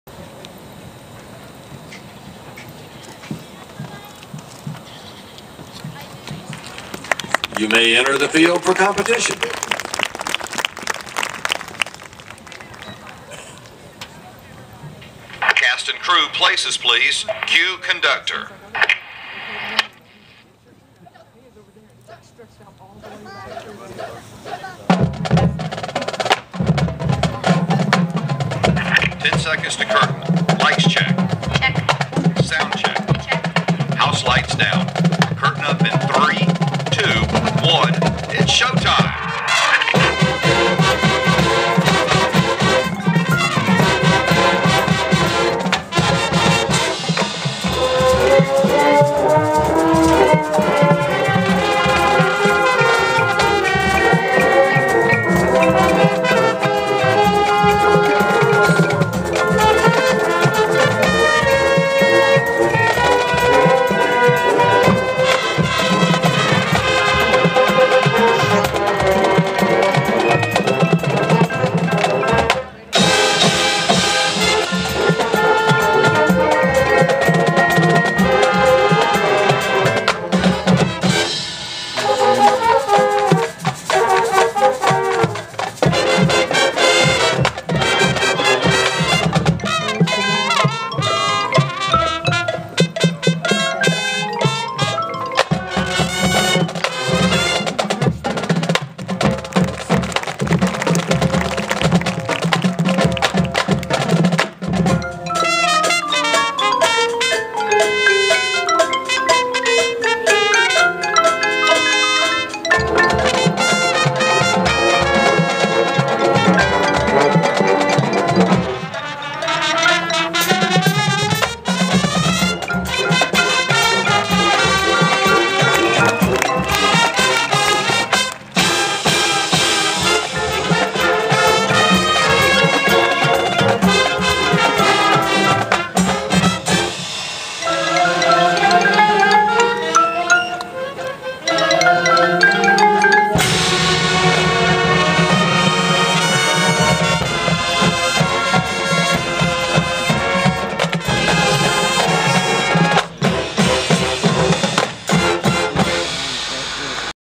Dalton High School Band - Pt 1
Dalton High School Band (Part 1) performing at the Blue Ridge Marching Festival at Fannin County High School - Oct 16th, 2010.